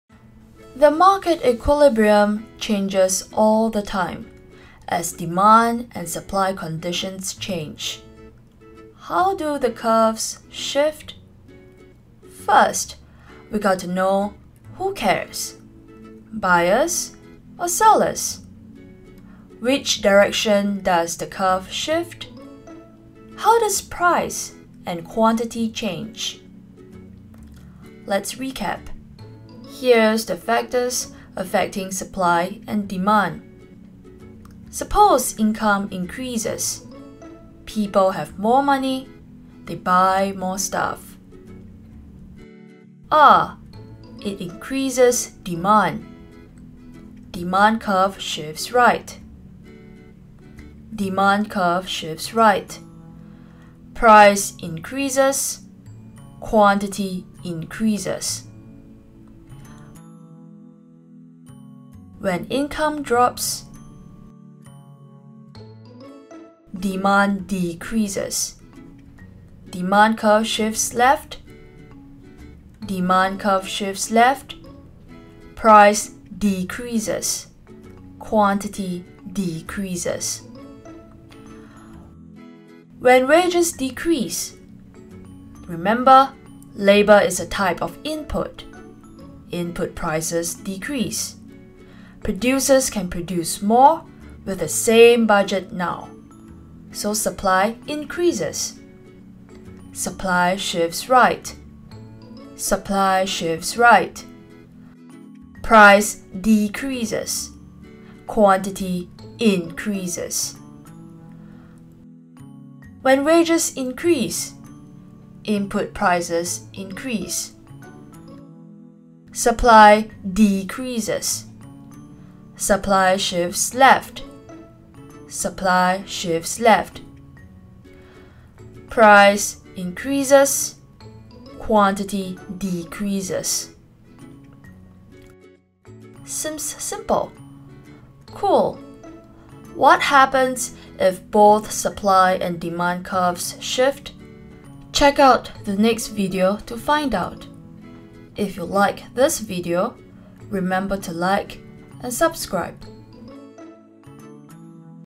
Shifting of Demand and Supply Curves (Part I)
Transcript:

1 The market equilibrium changes all the time
2 as demand and
3 supply conditions change.
How do the curves shift?
4 First, we gotta know who cares?
5 Buyers or sellers?
6 Which direction does the curve shift?
7 How does price and quantity change?

8 Let’s recap.
Here’s the factors affecting supply and demand.
9 Suppose income increases.
People have more money, they buy more stuff.

11 Ah…it increases demand.
13 Demand curve shifts right.
14 Demand curve shifts right.
15 Price increases.
16 Quantity increases.

19 When income drops,
21 Demand decreases.
23 Demand curve shifts left.
24 Demand curve shifts left.
25 Price decreases.
26 Quantity decreases.

29 When wages decrease,
Remember, labor is a type of input.
30 Input prices decrease,
Producers can produce more with the same budget now.
31 So supply increases.

33 Supply shifts right.
34 Supply shifts right.
35 Price decreases.
36 Quantity increases.

37 When wages increase,
38 Input prices increase.
39 Supply decreases.
41 Supply shifts left.
42 Supply shifts left.
43 Price increases.
44 Quantity decreases.

49 Seems simple.
Cool, what happens if both supply and demand curves shift?
Check out the next video to find out.
50 If you like this video,

Why does market equilibrium change all the time? Why do prices and quantities change all the time?

Well, market conditions change and those cause demand and supply curves to shift, affecting price and quantity. To determine which curve shifts, we need to understand who is the party here concerned? Sellers or buyers? Which direction does the curve shift? How do price and quantity change?

For instance, when more people prefer healthy good, demand for healthy food such as sushi and sandwiches increase, increasing price and quantity. There are other factors affecting demand too. For instance, price of substitutes and complements; future expectation of prices and population.

The supply curve shifts too. When natural disaster hits, factors of production get destroyed. It gets more expensive to produce car, for example. Hence, supply of cars decrease, the supply curve shifts left, causing price to increase and quantity to decrease.

In general, when demand increases, the demand curve shifts right. Price and quantity increase.
When demand decreases, the demand curve shifts left. Price and quantity decrease.
When supply increases, the supply curve shifts right. Price decreases and quantity increases.
When supply decreases, the supply curve shifts left. Price increases and quantity decreases.